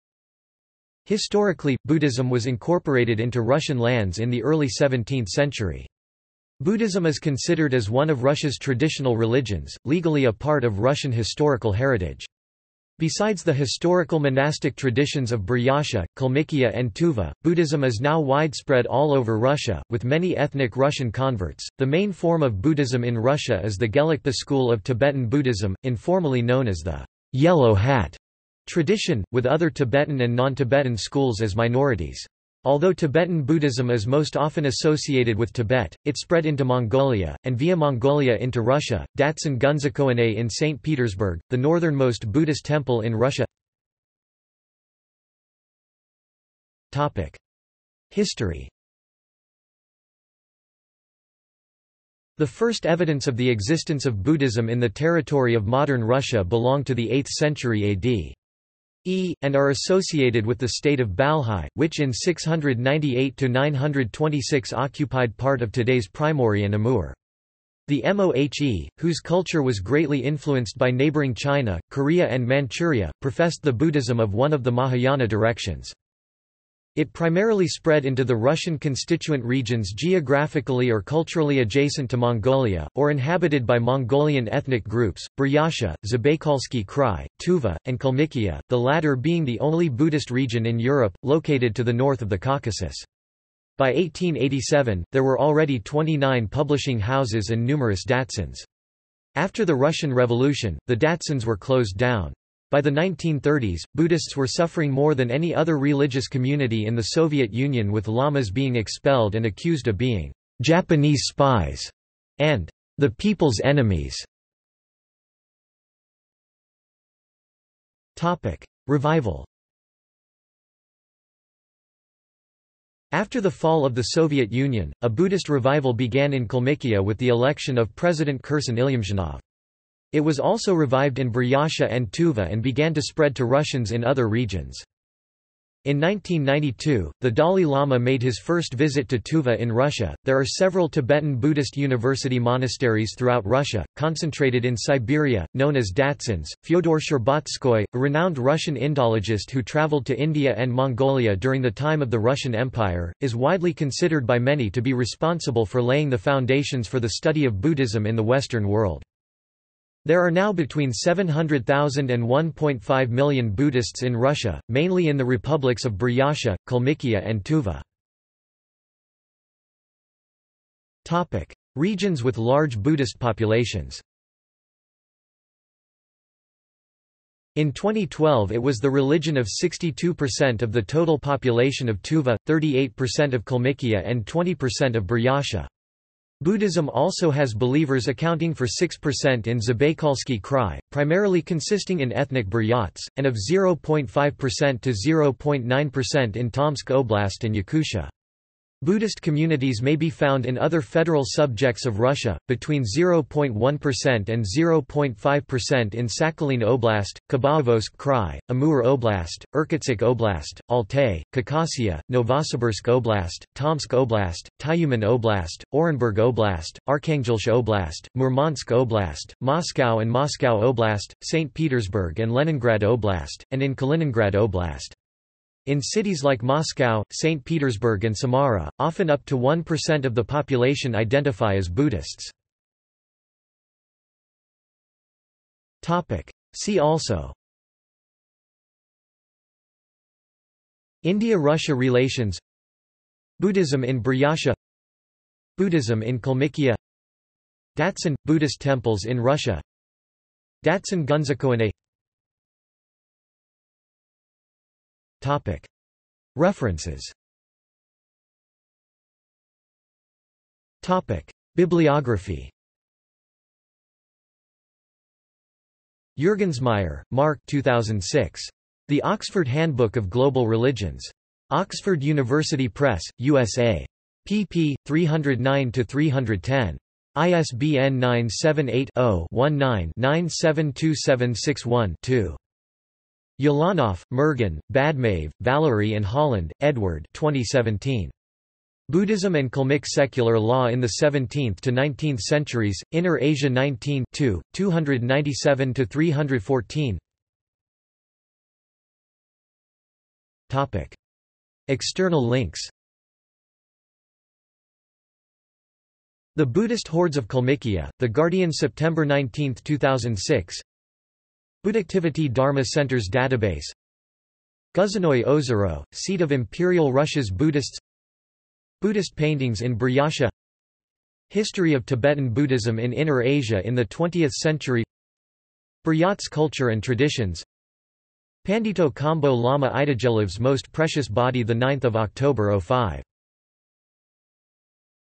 Buddhism in Russia | Wikipedia audio article
SUMMARY
Historically, Buddhism was incorporated into Russian lands in the early 17th century. Buddhism is considered as one of Russia's traditional religions, legally a part of Russian historical heritage. Besides the historical monastic traditions of Buryatia, Kalmykia and Tuva, Buddhism is now widespread all over Russia, with many ethnic Russian converts.The main form of Buddhism in Russia is the Gelukpa school of Tibetan Buddhism, informally known as the "yellow hat" tradition, with other Tibetan and non-Tibetan schools as minorities. Although Tibetan Buddhism is most often associated with Tibet, it spread into Mongolia, and via Mongolia into Russia.Datsan Gunzechoinei in Saint Petersburg, the northernmost Buddhist temple in Russia